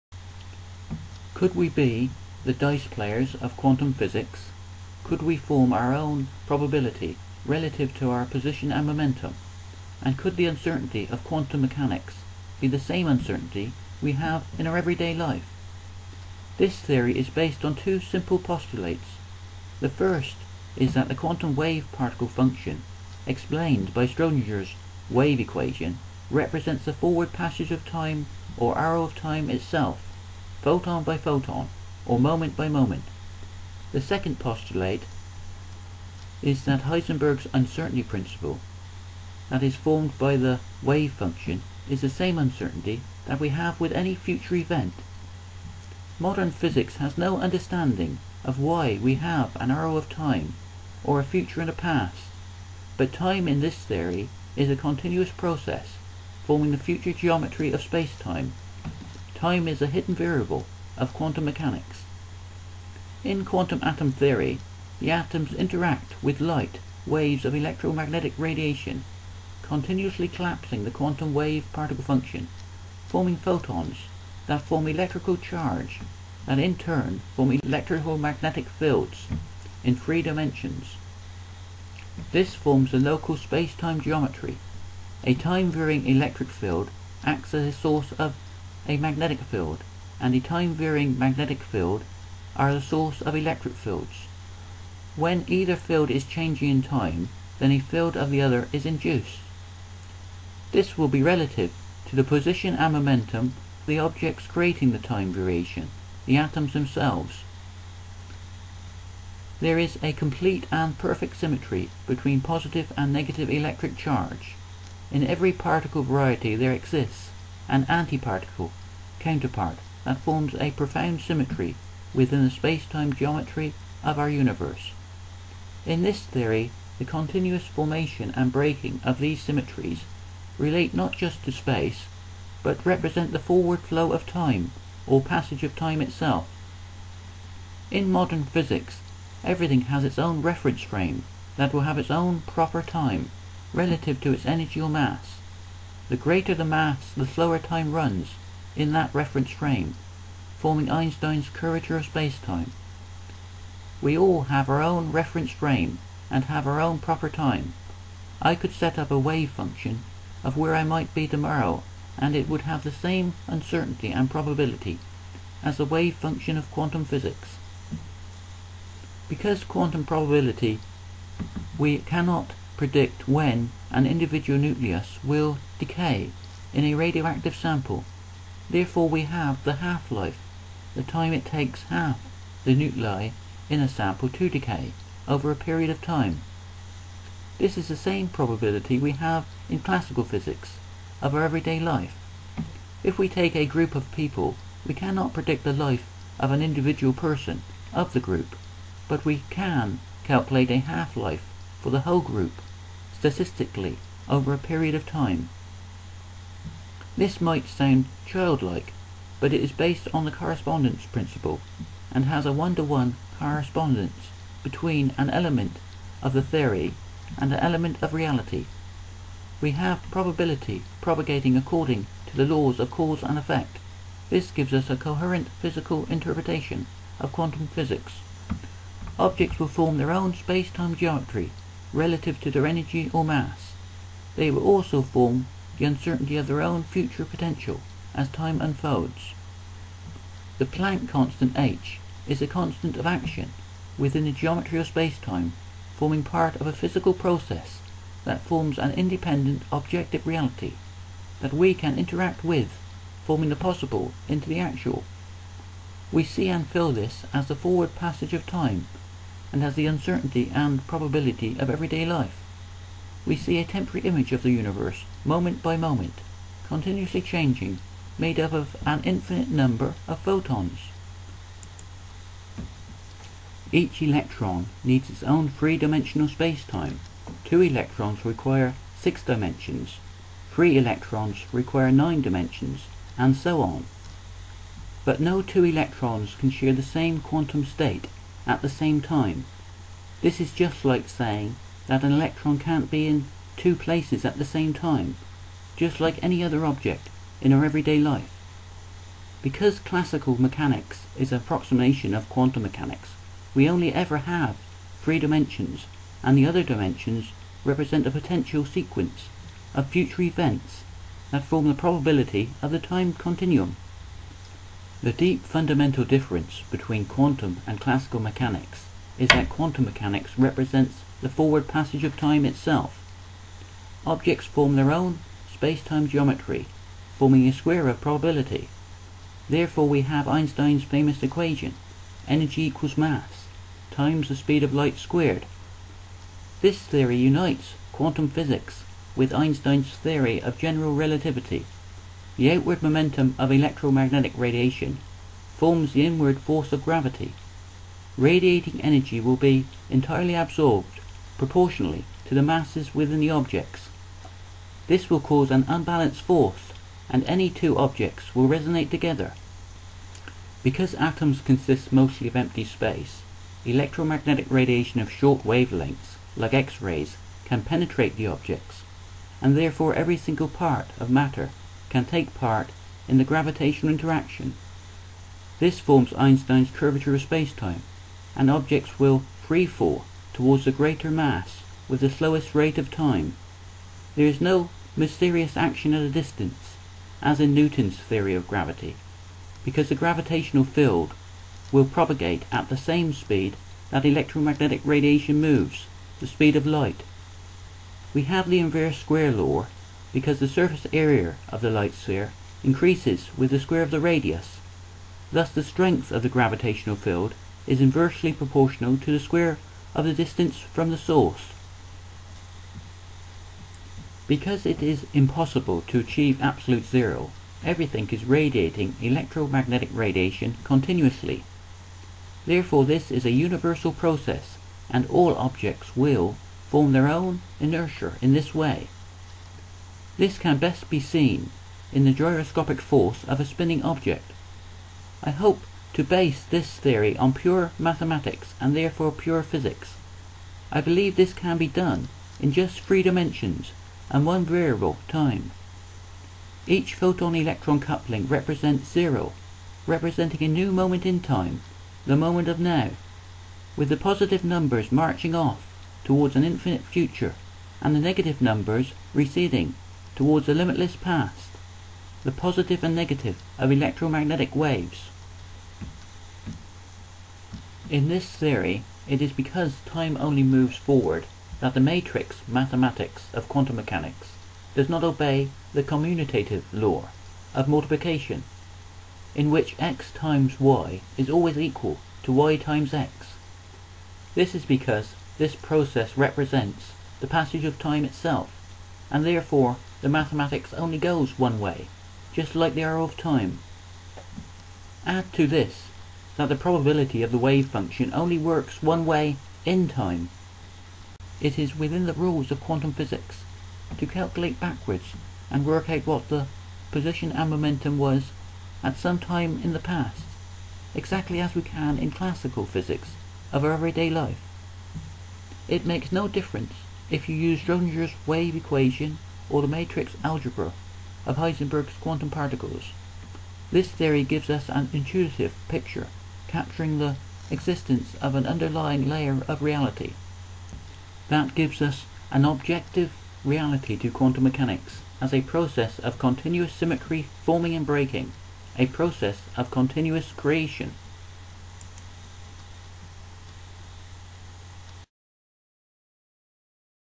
Are we the dice players of Quantum Physics?
Could we be the dice players of quantum physics, could we form our own probability relative to our position and momentum? And could the uncertainty of quantum mechanics be the same uncertainty we have in our everyday life.

This theory is based on two simple postulates,

1) The first is that the quantum wave particle function explained by Schrödinger's wave equation represents the forward passage of time or arrow of time itself photon by photon or moment by moment.

2) The second is that Heisenberg's Uncertainty Principle that is formed by the wave function is the same uncertainty that we have with any future event.

Modern physics has no understanding of why we have an arrow of time or a future and a past. But time in this theory is a continuous process forming the future geometry of spacetime; time is the hidden variable of quantum mechanics.